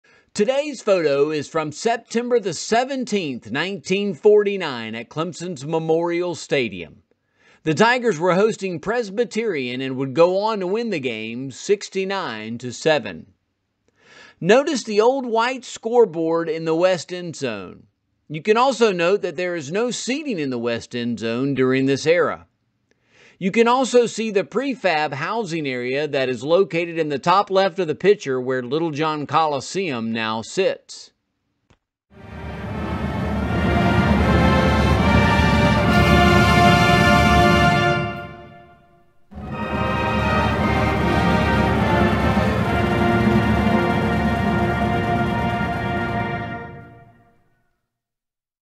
June 13th Clemson Historic Picture Of The Day
Today’s photos are from September 17th, 1949 at Clemson’s Memorial Stadium. The Tigers were hosting Presbyterian, and would go on to win the game 69-7.

The Tigers were embarking on the 1949 season where we would finish 4-4-2 with a 2-2 record in the Southern Conference. Leading the Tigers on the field in 1949 was the legendary Fred Cone. This photo was taken from the South Stands facing towards the North Stands.

Notice the old white scoreboard in the west endzone. You can also note that there is no seating in the west endzone during this era. You can also see the prefab housing area that is located in the top left of the picture (where Littlejohn Coliseum now sits).

I also chuckle a bit when I see this picture because it appears several cars literally pulled up to the bank on the northwest endzone area and parked before heading into the game! You can also note that the North Stands had not gone through its expansion in this picture. The top of the North Stands in this picture is where the portals are now at Memorial Stadium.

In 1958, 18,000 sideline seats were added (the “above the portals area” of the North and South Stands) and in 1960, 5,658 west end zone seats were added in response to increasing attendance. The original cedar wood seating was replaced in 1972 by aluminum seats. As attendance continued to skyrocket, an upper deck was added to each side of the stadium.